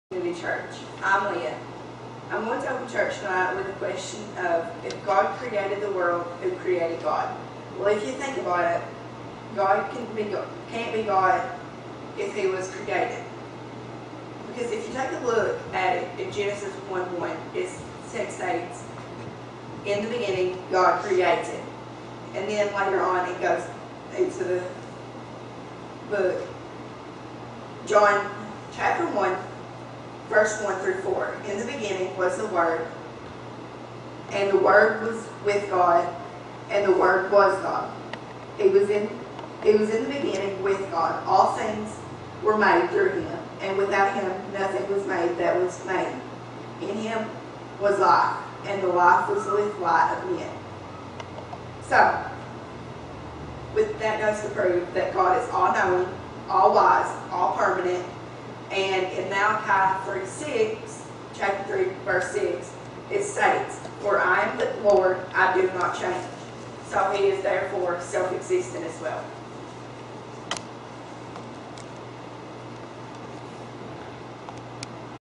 Who Created God?
Check out one of our teens as they give an unapologetic answer to an apologetic question many have asked! Ms. Leah Peters did a great job!